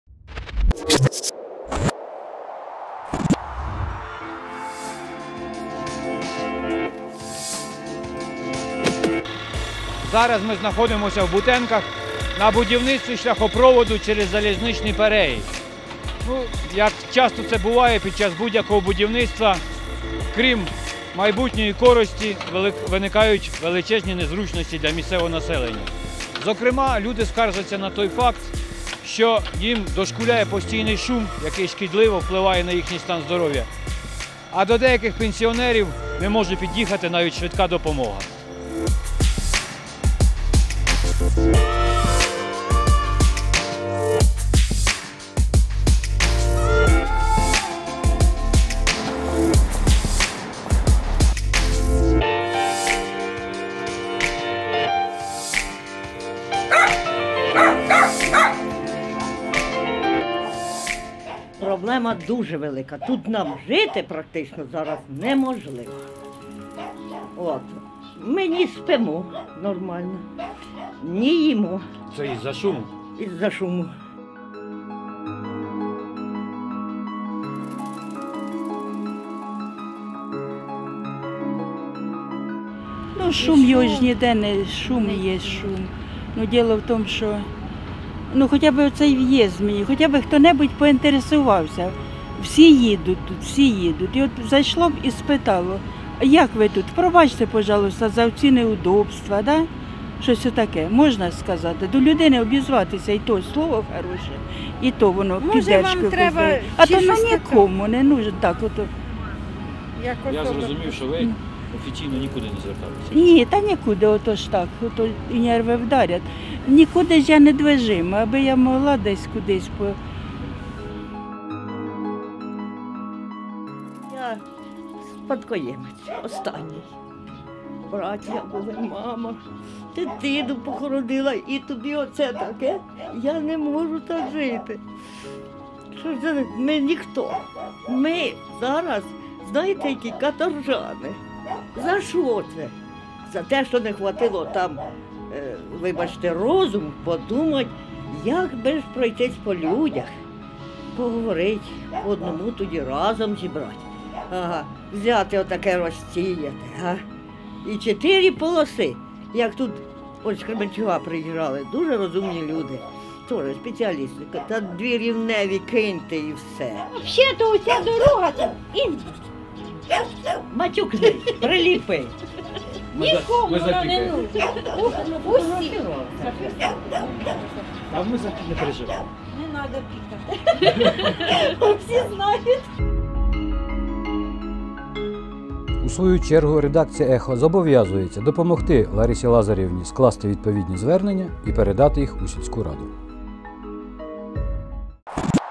Дорогу будують, голови болять
Зараз у Бутенках триває грандіозне і довгоочікуване будівництво нової дороги, що з’єднає Дніпро та Решетилівку. Зараз шляховики споруджують гігантську розв’язку на перетині двох важливих автодоріг та шляхопровід через залізничний переїзд. І нібито всі мають лише радіти з цього приводу. Але дехто з мешканців Бутенок говорить, що може просто не дожити до завершення робіт, потерпаючи від постійного шуму будівельних механізмів.